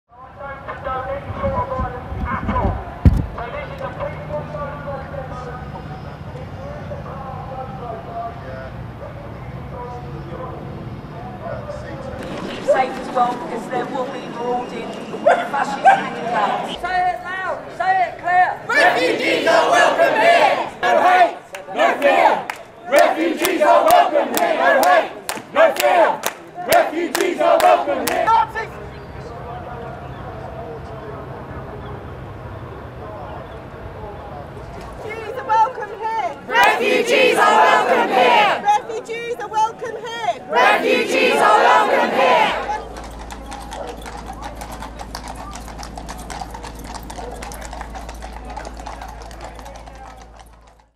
Pro-refugee and anti-immigration groups protest in Dover
Anti-migrant protesters have clashed with police and blocked a dual carriageway into Dover, whilst pro-refugee groups have assembled nearby.

At least one anti-migrant protestor has been arrested.

Dover continues to be a prime entry for migrants attempting to access the country.

Over 1,450 migrants arrived in the UK by small boats in August - believed to be the record for a single month - according to reports.